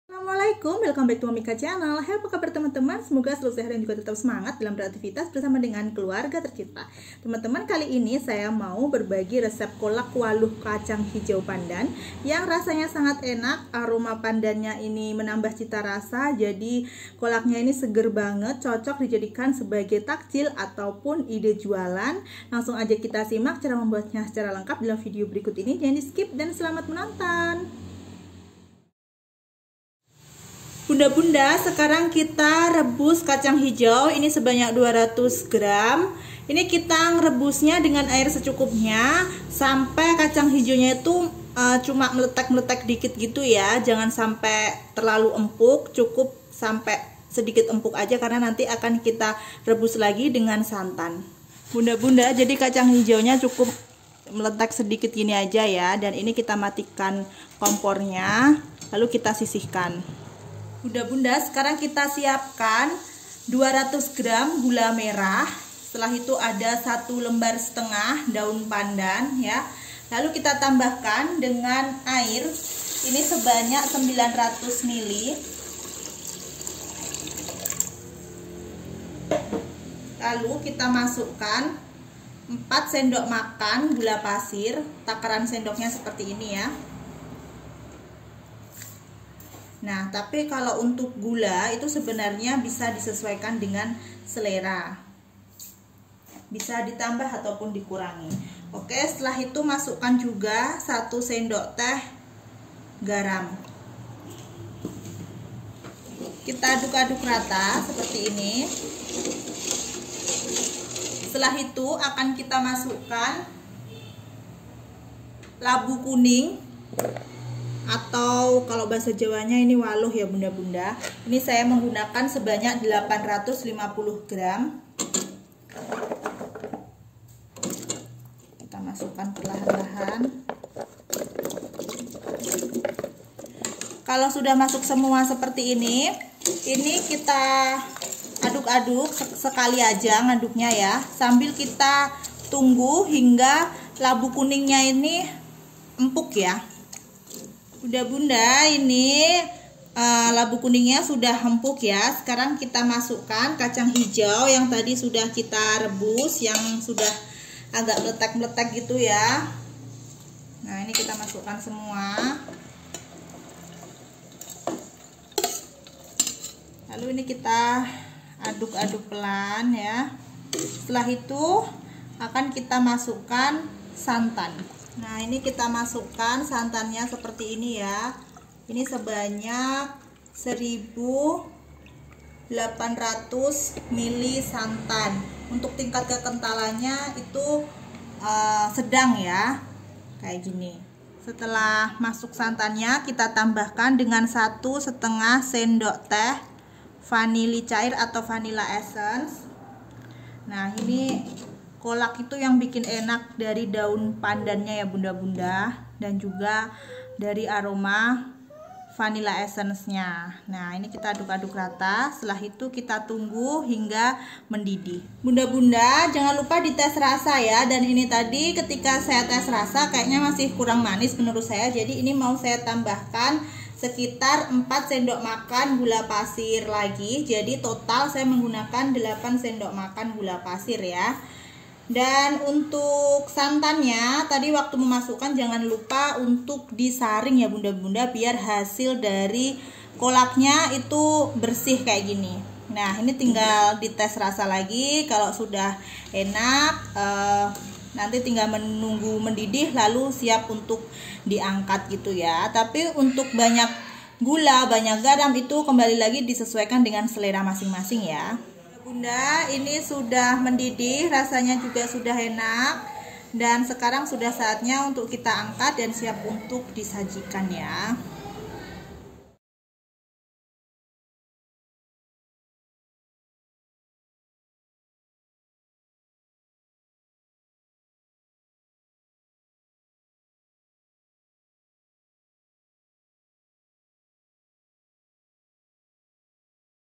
RESEP KOLAK WALUH KACANG HIJAU PANDAN YANG ENAK
Resep Kolak Waluh Kacang Hijau Pandan :
- 850 gr waluh atau labu kuning
- 200 gr kacang hijau
- 900 ml air
- 1800 ml santan
- 8 sdm gula pasir
- 200 gr gula merah
- 1½ lembar daun pandan
- 1 sdt garam
- 1 sdt vanilli cair atau vanilla esense